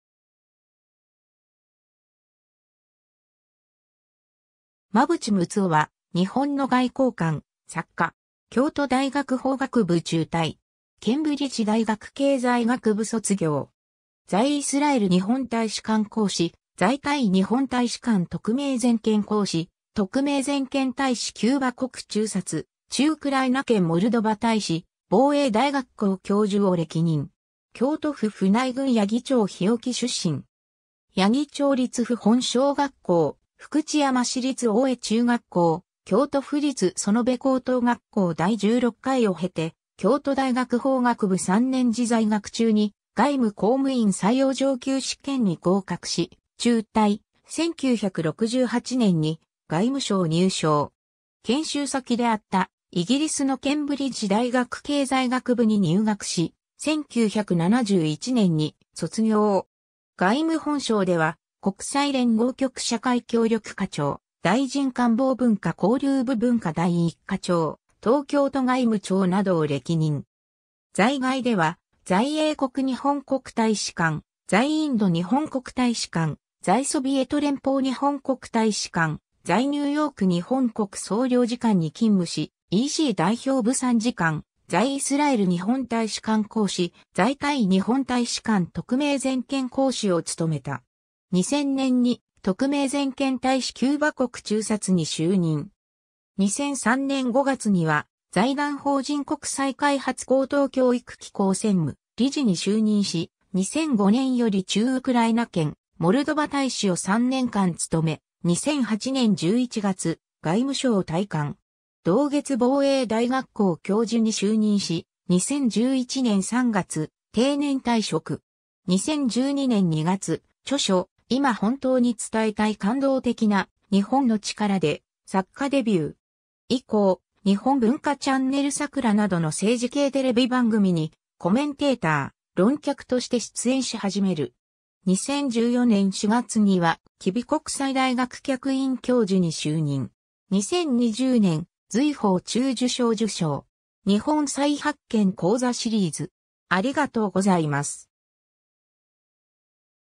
馬渕睦夫
馬渕 睦夫（まぶち むつお、1946年（昭和21年） - ）は日本の外交官、作家。
 京都大学法学部中退。
ケンブリッジ大学経済学部卒業。
在イスラエル日本大使館公使、在タイ日本大使館特命全権公使、特命全権大使キューバ国駐箚、駐ウクライナ兼モルドバ大使、防衛大学校教授を歴任。
 京都府船井郡八木町日置出身。
八木町立富本小学校、福知山市立大江中学校、京都府立園部高等学校第16回を経て、京都大学法学部3年次在学中に外務公務員採用上級試験に合格し中退、1968年（昭和43年）に外務省入省。
 研修先であったイギリスのケンブリッジ大学経済学部に入学し、1971年（昭和46年）に卒業。
 外務本省では、国際連合局社会協力課長（1984年-1986年）、大臣官房文化交流部文化第一課長、東京都外務長（1995年-1997年）などを歴任。
 在外では、在英国日本国大使館、在インド日本国大使館、在ソビエト連邦日本国大使館、在ニューヨーク日本国総領事館に勤務し、EC代表部参事官（1989年-1991年）、在イスラエル日本大使館公使（1991年-1995年）、在タイ日本大使館特命全権公使（1997年-2000年）を務めた。
 2000年（平成13年）に特命全権大使キューバ国駐箚に就任。
2003年（平成15年）5月には財団法人国際開発高等教育機構専務理事に就任し、2005年（平成17年）より駐ウクライナ兼モルドバ大使を3年間務め、2008年（平成20年）11月、外務省退官。
同月防衛大学校教授に就任し、2011年（平成23年）3月、定年退職。
 2012年（平成24年）2月、著書『いま本当に伝えたい感動的な「日本」の力』で作家デビュー。
以降、日本文化チャンネル桜などの政治系テレビ番組（主に外交・国際政治関連）にコメンテーター・論客として出演し始める。
2014年（平成26年）4月には吉備国際大学客員教授に就任。
 2020年、瑞宝中綬章受章。
 「日本再発見」講座シリーズ